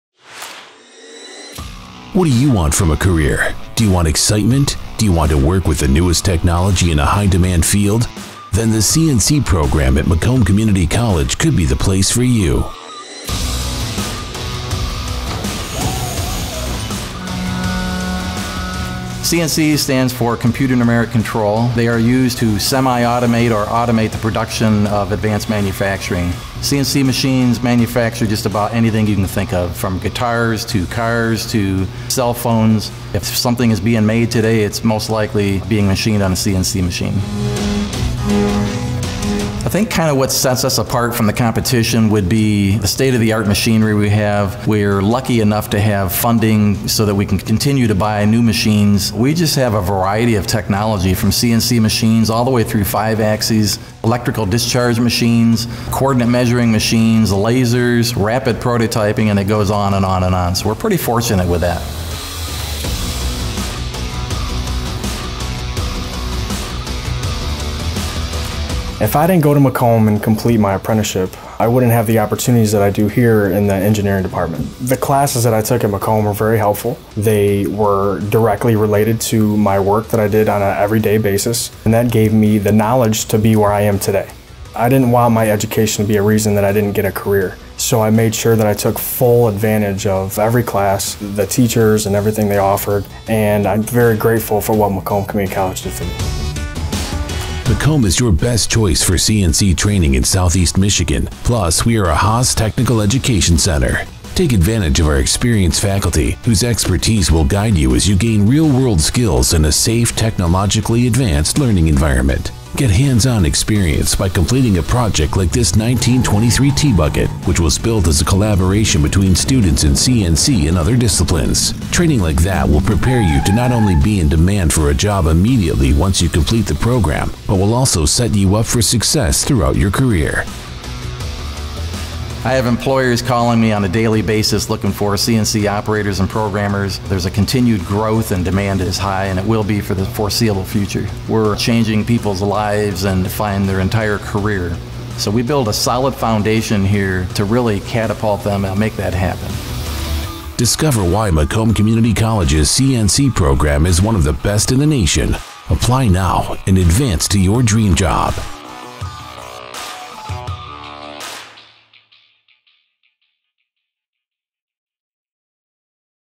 Applied Technology - Advanced Processes (CNC) at Macomb
Macomb Community College offers state-of-the-art degree and certificate programs in many advanced processes, including Computer Aided Design (CAD), Computer Aided Manufacturing (CAM) and Computer Numerical Control (CNC). Get real-world training that will prepare you to be immediately employable and set you up for success throughout your career.